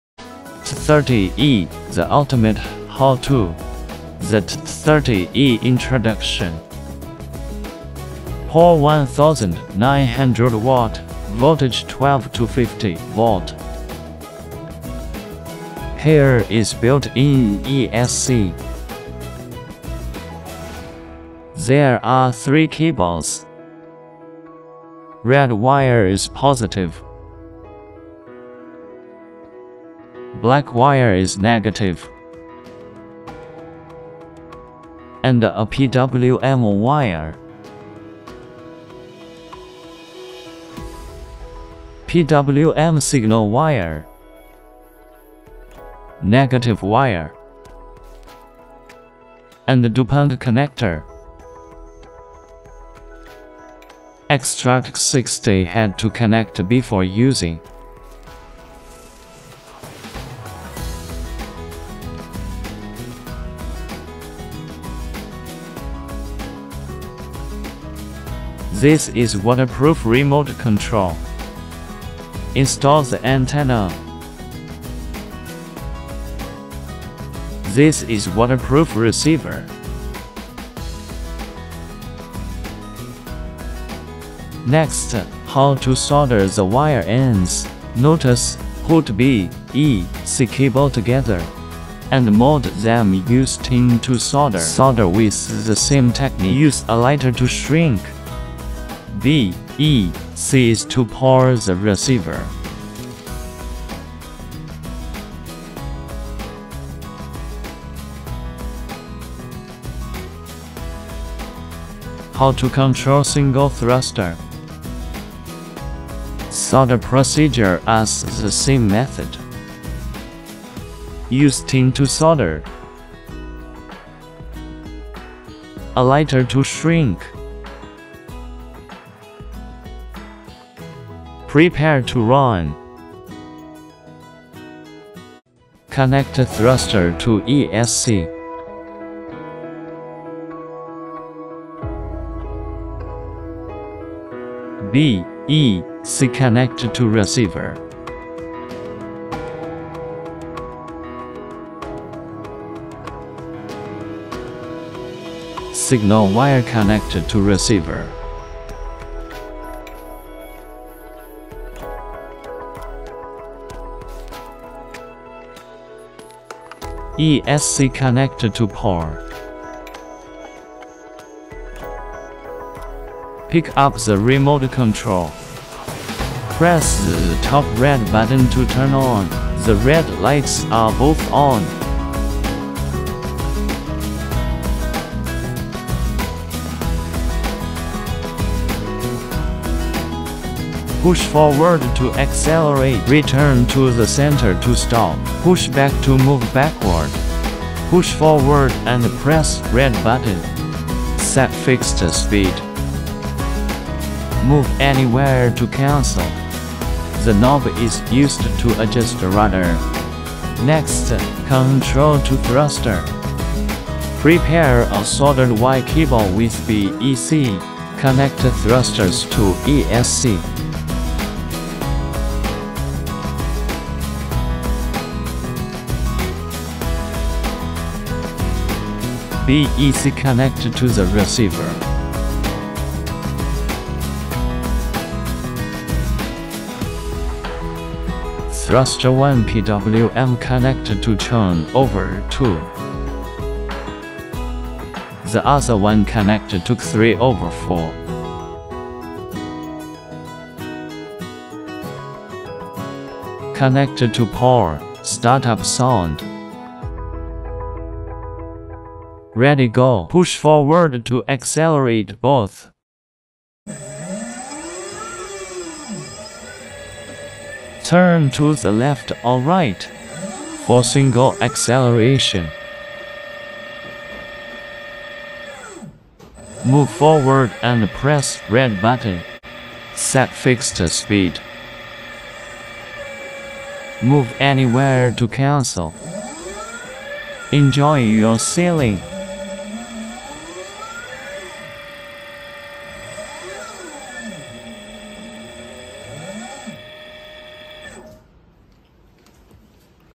TD30E Thruster: Easy How-To Guide!
Watch this easy guide on using the TD30E thruster! We show you how to solder, set up the remote, and more. Great for beginners and anyone wanting to master the TD30E.